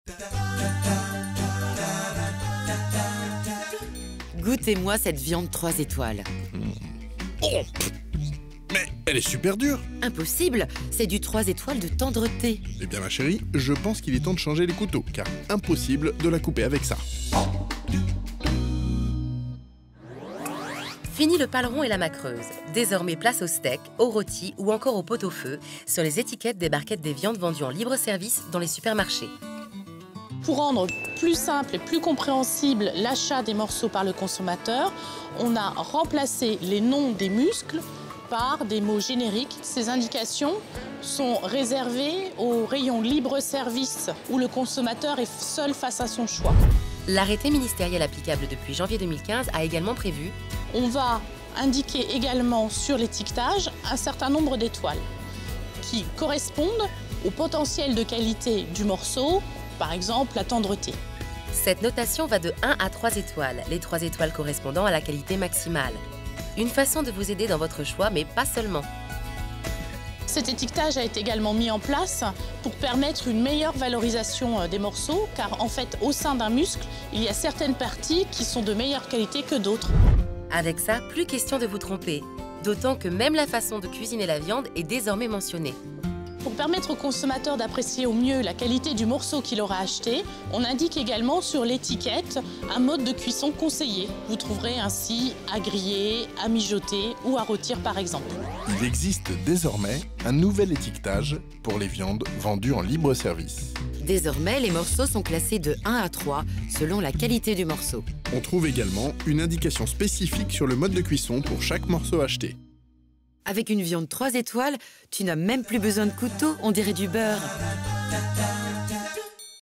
Consomag : "Un nouvel étiquetage pour les viandes !"
De nouvelles étiquettes ont vu le jour sur les barquettes de viandes, voici comment les déchiffrer !